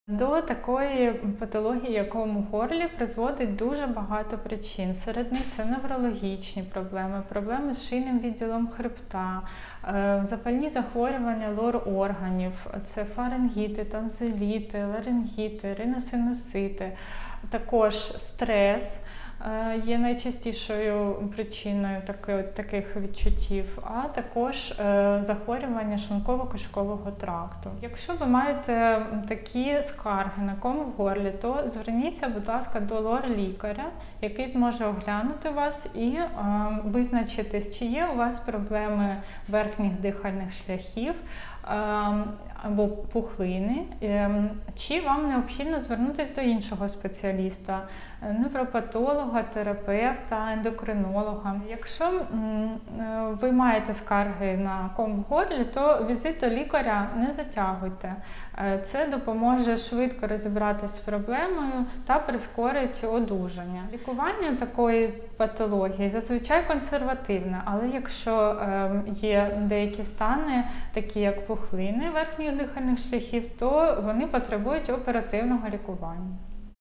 Відчуття кому в горлі
До відчуття кому в горлі можуть призвести багато причин. Детальніше про них розповідає лікар-отоларинголог Ксенія Хмельницька.

🏥 Smart Medical Center — сучасний медцентр для всієї сім’ї